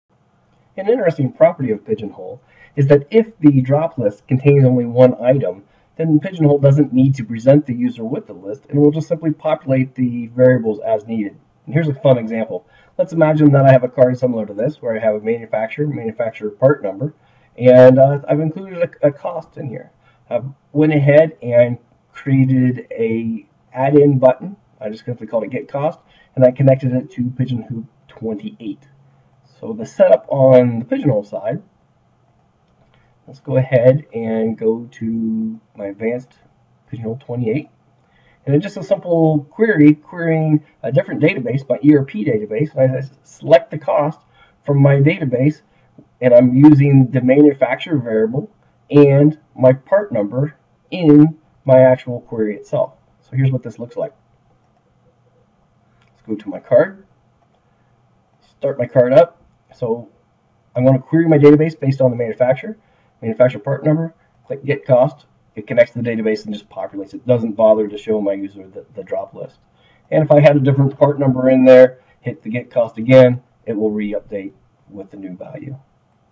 PigeonHole Updating Data Cards by Clicking a Button
PigeonHole can update a SolidWorks Enterprise PDM data card's information just by clicking a card button.